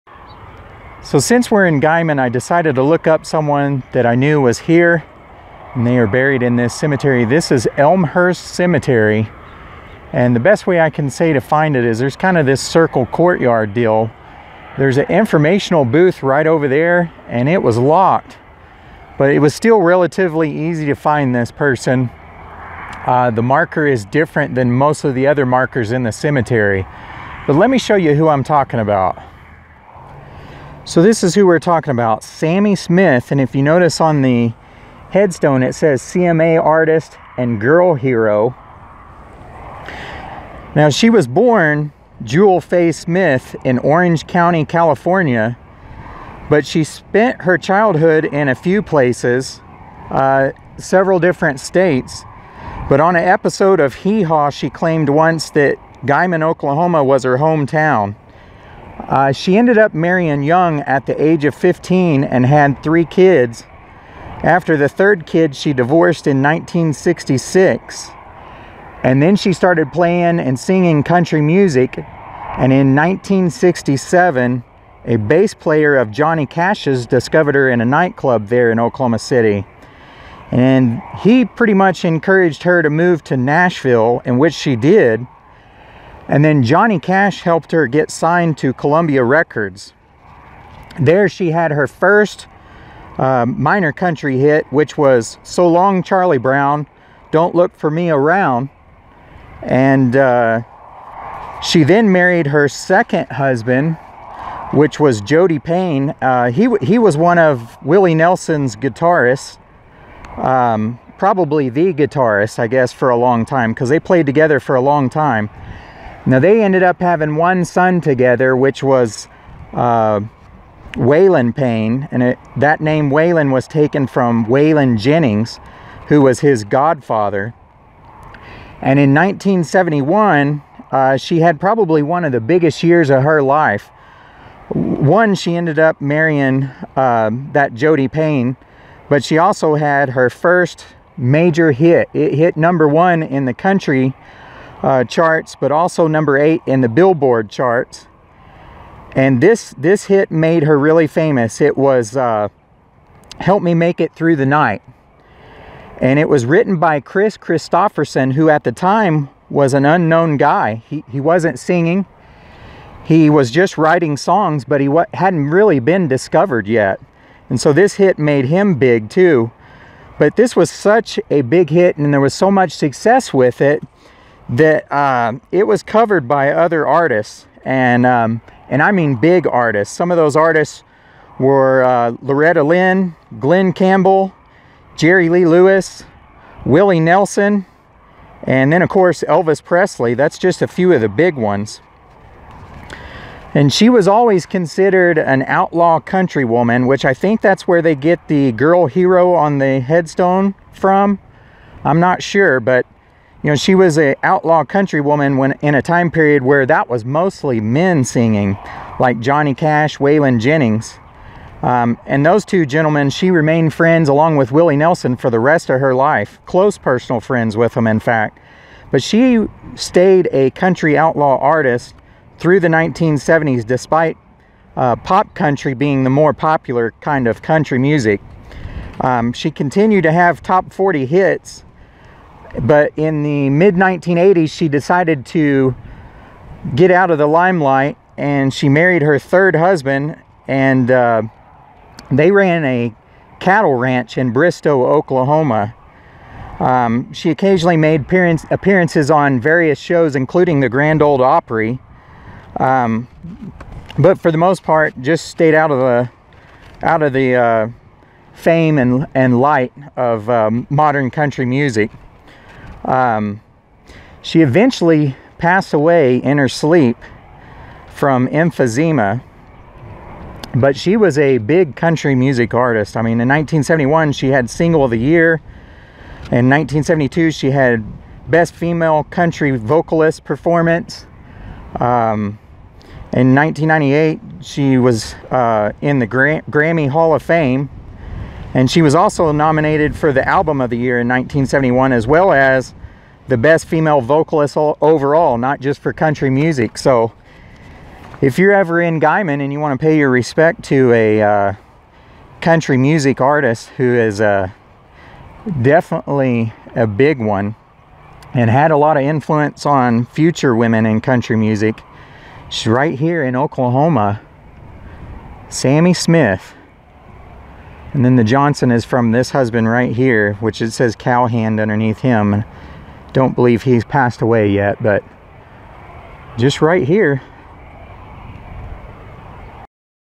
Outlaw Country Artist Sammi Smith's Final Resting Place
Sammi was part of the Outlaw Country Music movement and really the only successful female one.